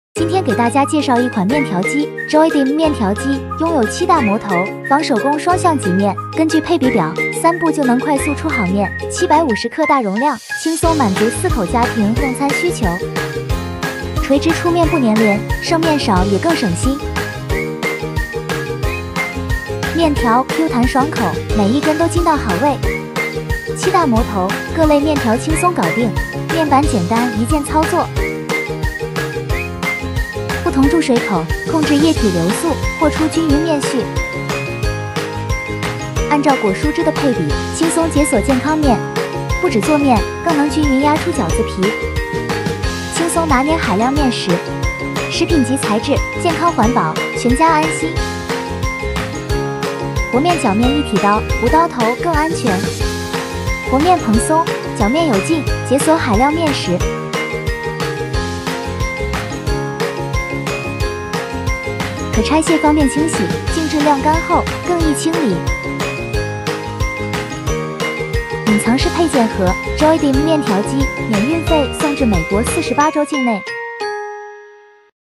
多亏Joydeem面条机，在家3步就可以吃到新鲜面条啦
Joydeem面条机介绍如下：
1.根据配比表，3步快速出好面，筋道爽口
2.7大模头，能做7种面型，解锁海量面食食谱
3.垂直90°出面，根根分明，不粘黏剩面少
4.750g大容量，满足1-4人家庭，与家人大口嗦面
5.和面搅面一体刀，可拆组件，方便清洗
6.隐藏式配件盒，方便收纳，使用随心
7.标准120V电压，免运费送至美国48州境内